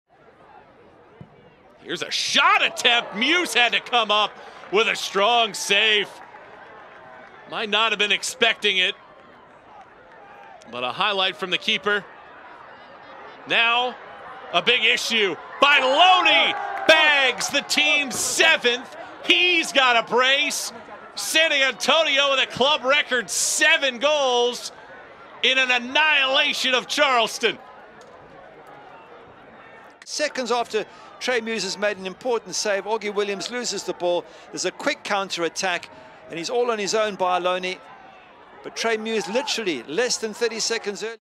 Ignacio Bailone with a Goal vs. Charleston Battery
Ignacio Bailone (San Antonio FC) with a Goal vs. Charleston Battery, 05/14/2023